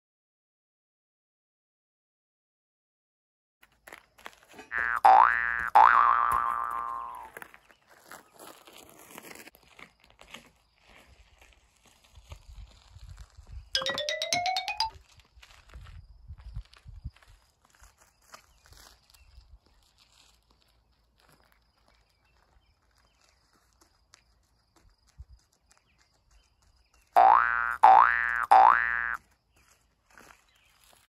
DIY unicycle course
I made this course out of metal and wood scrapes you can come up with lots of ideas with left over housing supplies.